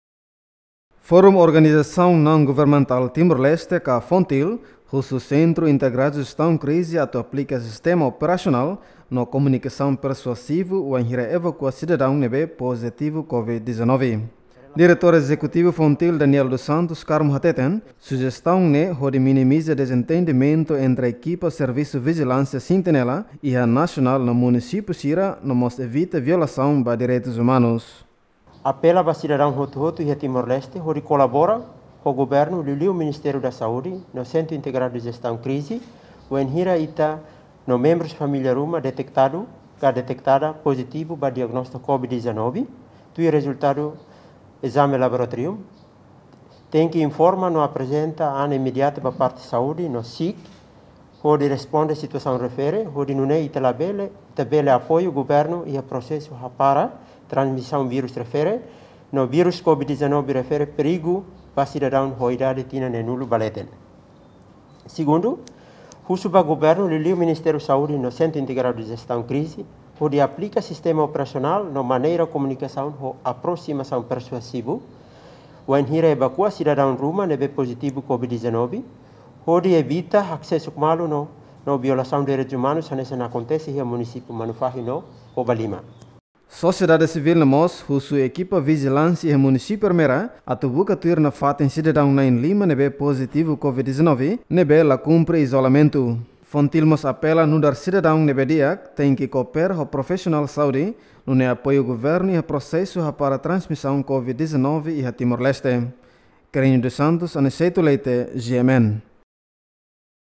FONGTIL sujere ba SIJK atu hadia sistema evakuasaun pasiente COVID 19
FONGTIL sujere ba Sentru Integradu Jestaun Krize atu  hadia sistema evakuasaun pasiente Covid 19